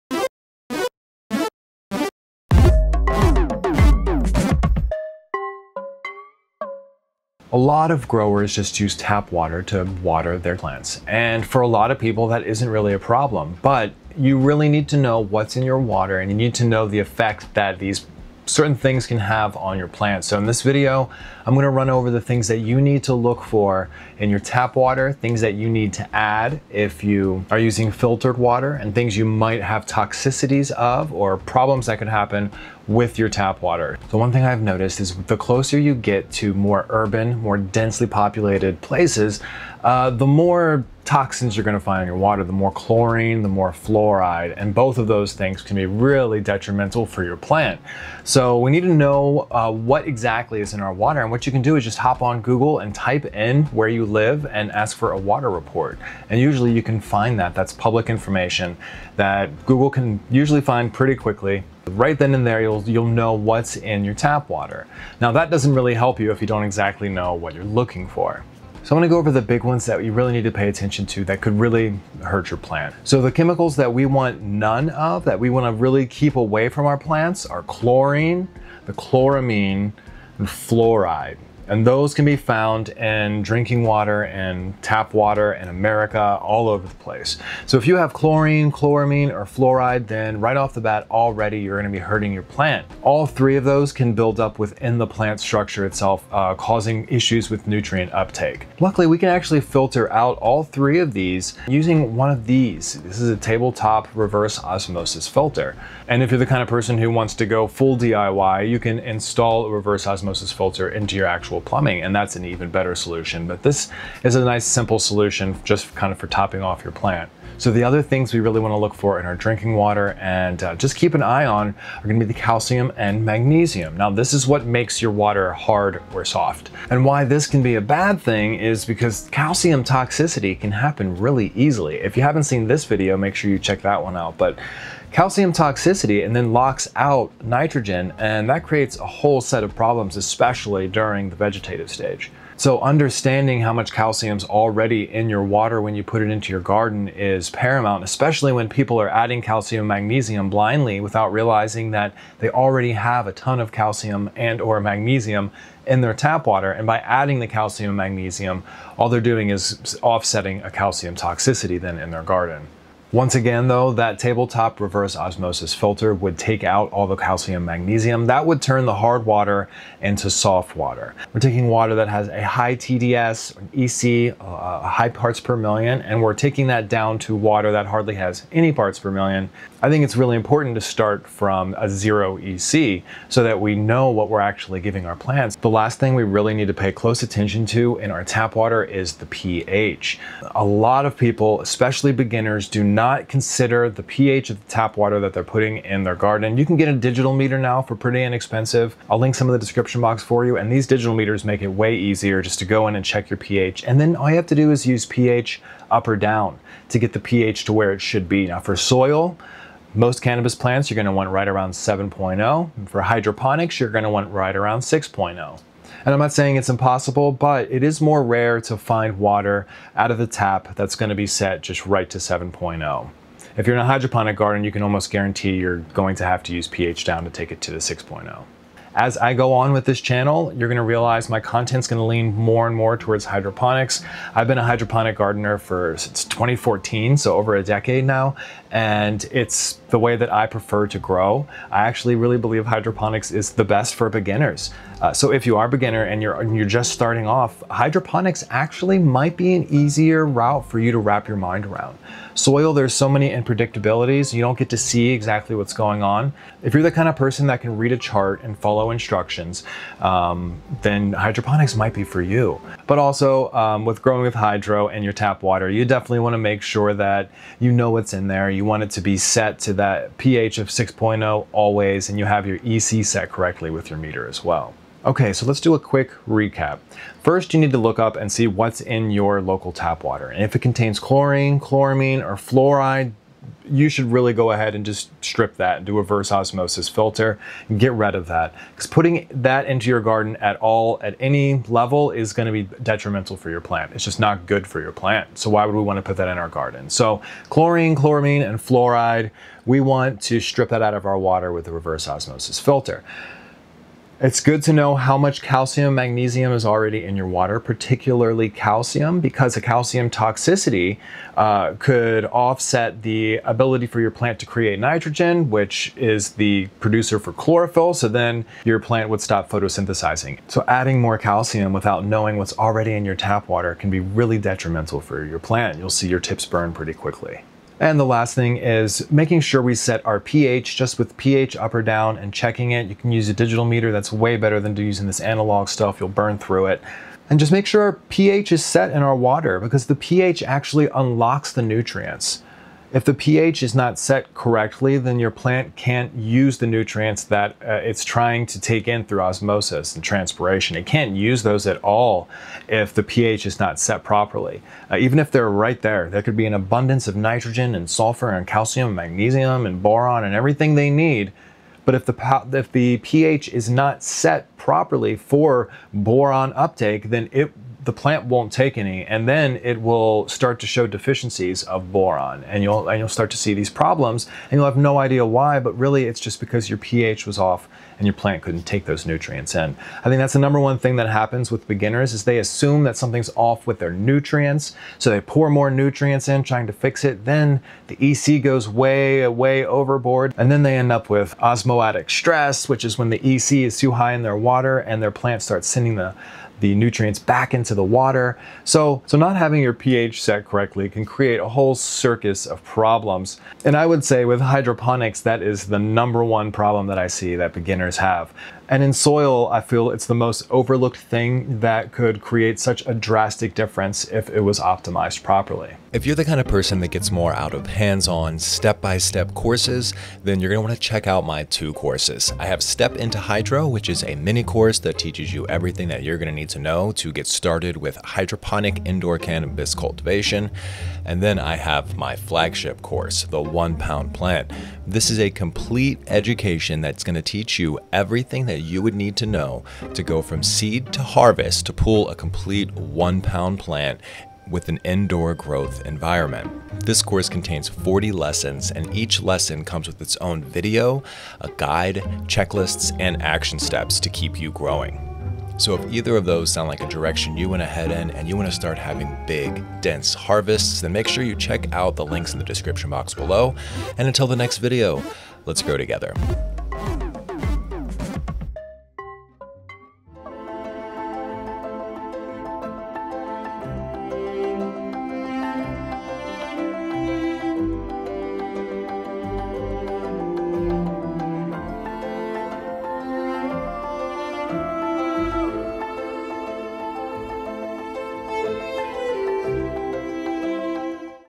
Is Tap Water Safe For Plants? 3 Steps To Get Your Plants To LOVE Your Tap Water
Tabletop RO Fileter

Full RO Filter System

PH Meter

PH up and down

Chad's Grow Room Skool Community (where you find all the free guides)

Stop guessing... These simple steps can take your plant from droopy and sad to thriving and vibrant. If you are wandering if you can use tap water for you plants, well, the answer is, maybe. Learn what to look for and how to treat your water properly so that you plants will love it and your nutrient uptake will be optimized. So many people just water from the sink, and never know that they are hurting their overall yield. By putting Fluoride, Chlorine, Chloramine or an abundance of Calcium and Magnesium. Optimizing your water is the first place to start in your quest for a 1lb plant.